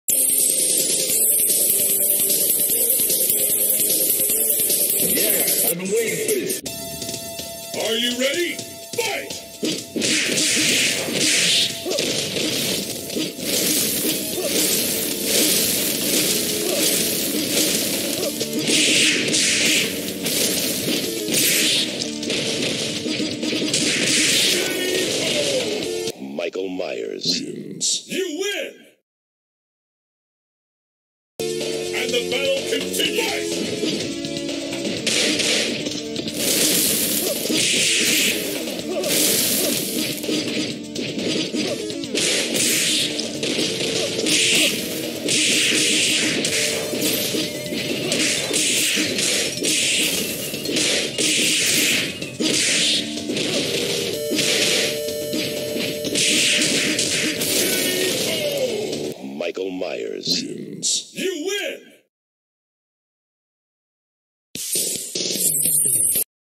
MMV Halloween event trick or treat coming soon...
Showing the last bit of Michael Myers minor color separation. I haven't added anything else new as I have been busy with other releases and the event.